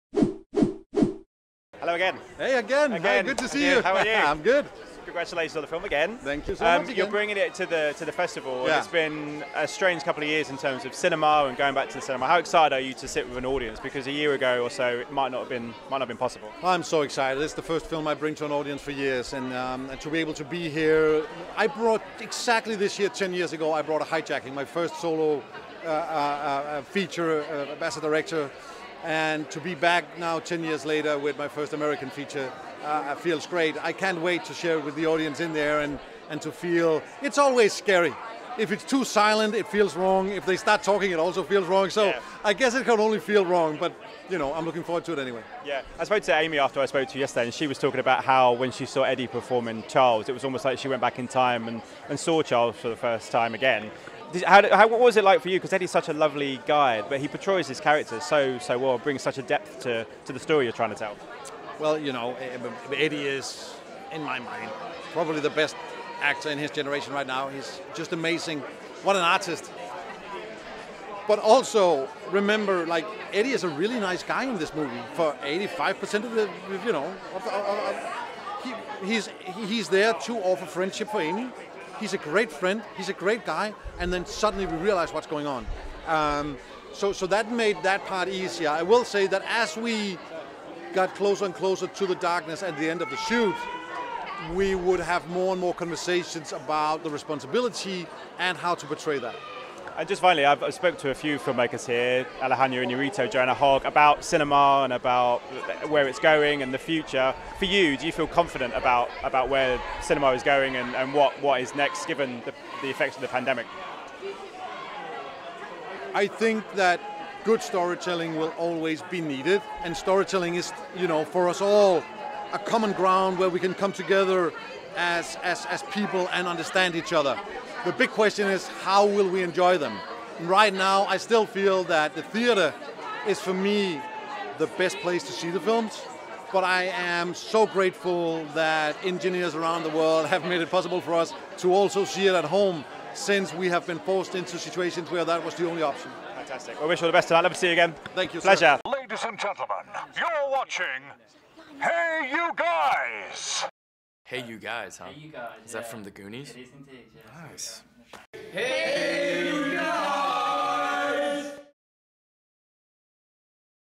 The Good Nurse LFF Premiere - Tobias Lindholm on the darkness at the heart of his power new film
Tobias Lindholm is interviewed at The Good Nurse premiere at the 2022 London Film Festival. Released by Netflix and directed by Tobias Lindholm, the film stars Jessica Chastain, Eddie Redmayne, Nnamdi Asomugha, Kim Dickens, Malik Yoba, Alix West Lefler and Noah Emmerich. Colin Hart and Scott Davis were on the red carpet.

Plot: Amy, a compassionate nurse and single mother struggling with a life-threatening heart condition, is stretched to her physical and emotional limits by the hard and demanding night shifts at the ICU. But help arrives, when Charlie, a thoughtful and empathetic fellow nurse, starts at her unit. While sharing long nights at the hospital, the two develop a strong and devoted friendship, and for the first time in years, Amy truly has faith in her and her young daughter’s future. But after a series of mysterious patient deaths sets off an investigation that points to Charlie as the prime suspect, Amy is forced to risk her life and the safety of her children to uncover the truth.

The film hits select cinemas on October 19th, hitting the streamer globally on October 26th.